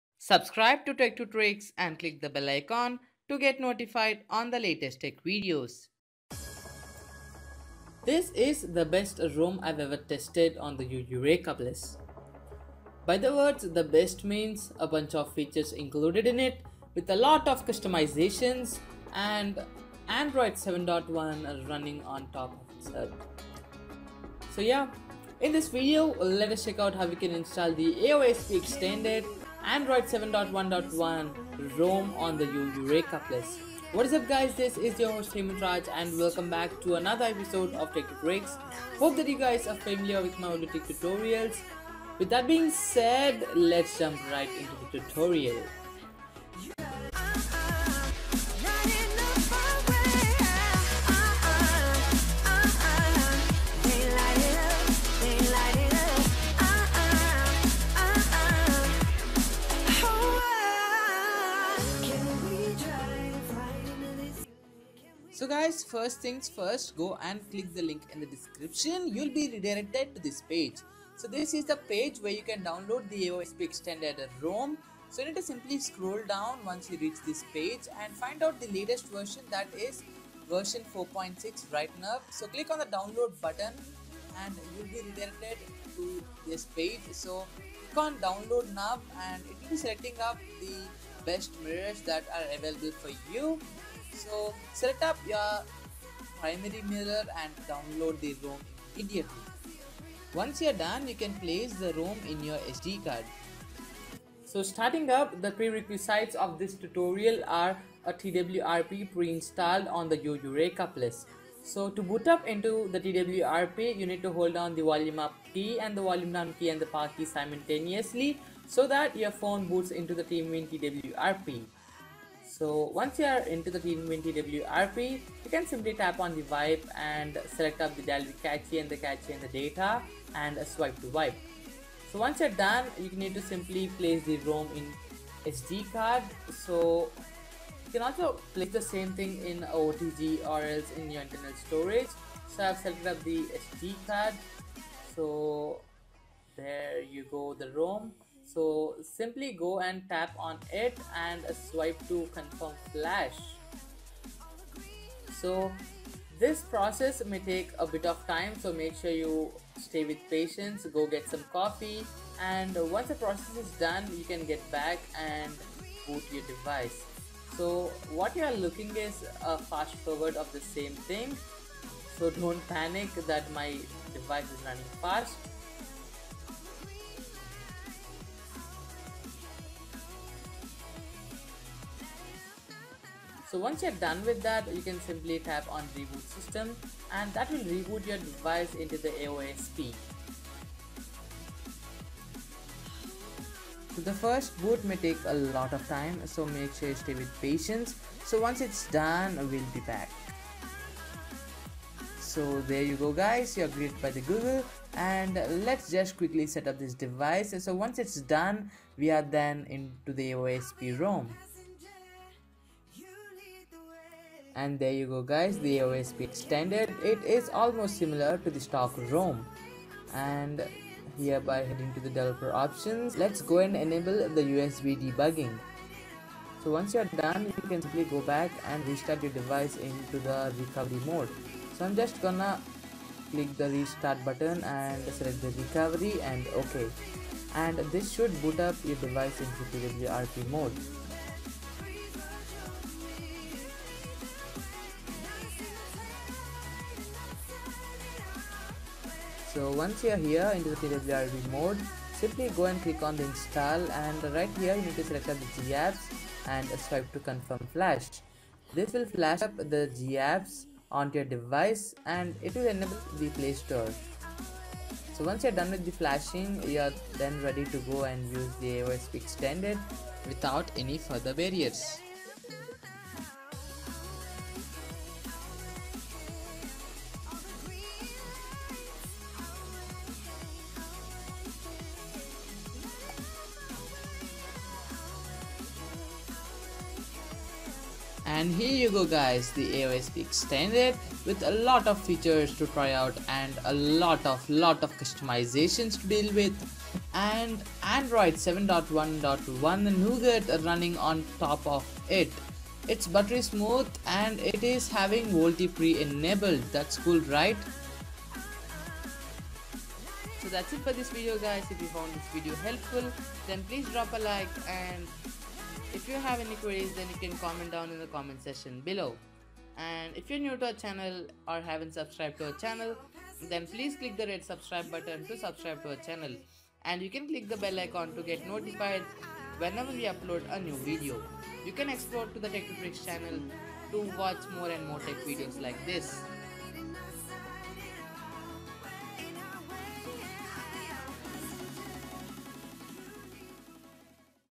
Probably the best Rom ive ever tested on Yu Yureka Plus {Official Rom}
In this video we have described how we can install the aosp extended rom